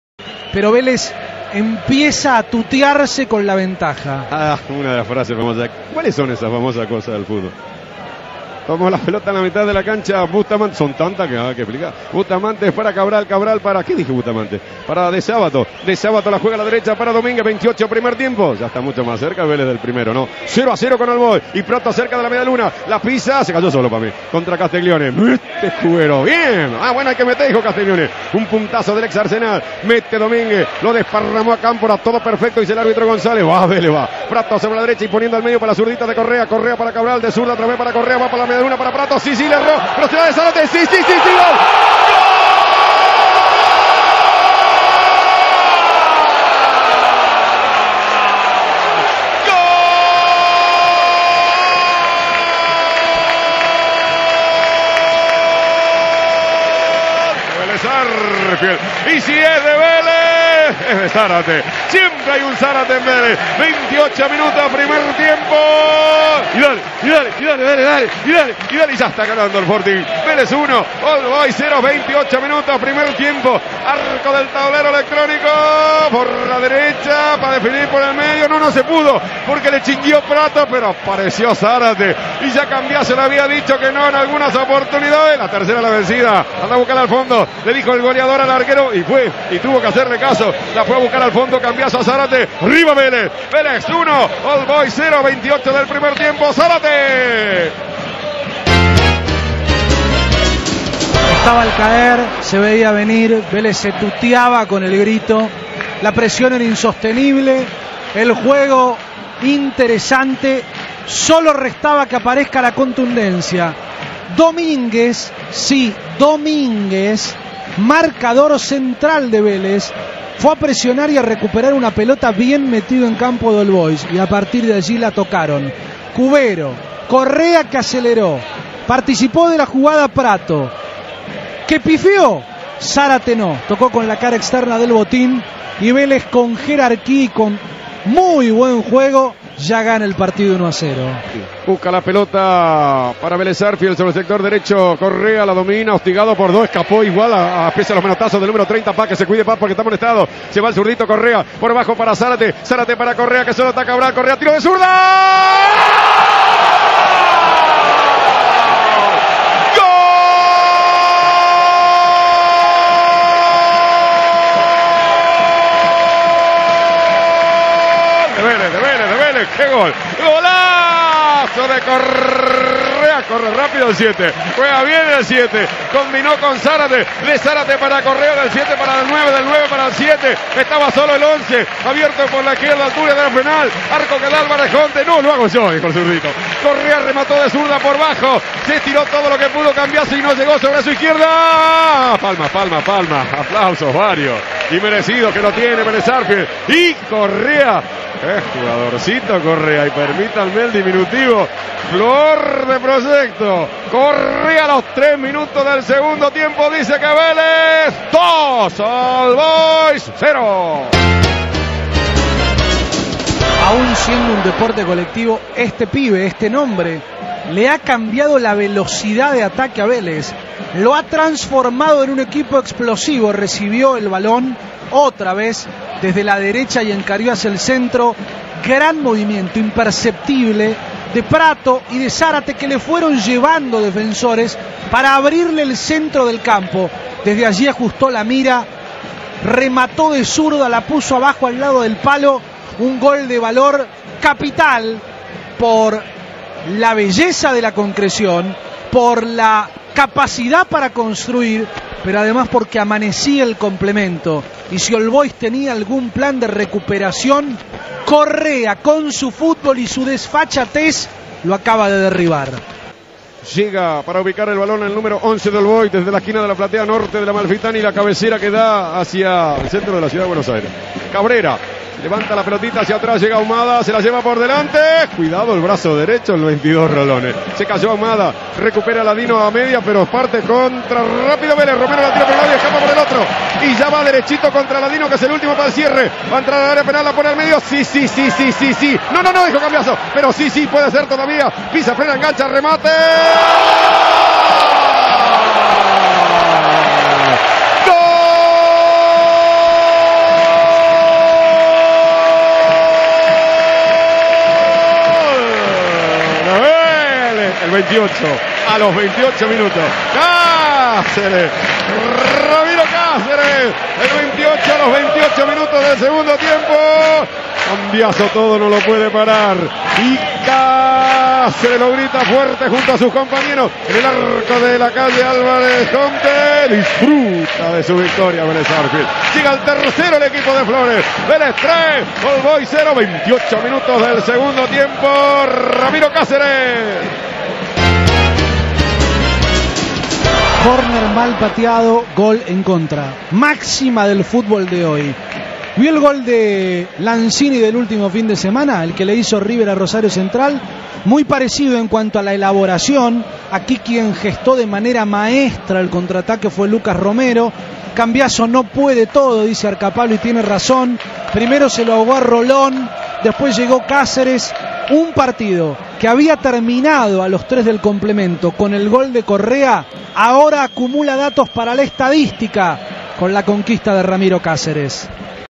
Velez 3 All Boys 0 (Relato Jorge Arcapalo) Torneo Final 2014 Los goles
Los goles del partido con relato de Jorge Arcapalo Am 590 Radio Continental